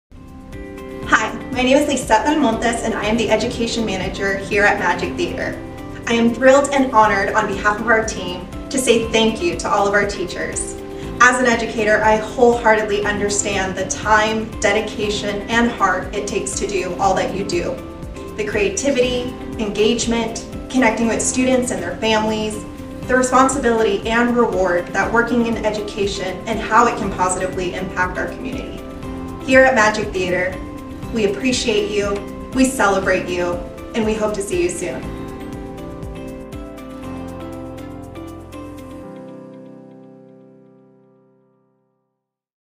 Happy Teacher Appreciation Week from Magik Theatre!
As a token of appreciation for the remarkable impact that educators have on young people's lives, Magik Theatre is proud to offer classroom resources and an exclusive discount on tickets for teachers. This is our way of saying thank you for your dedication, passion, and commitment to shaping the future of our community.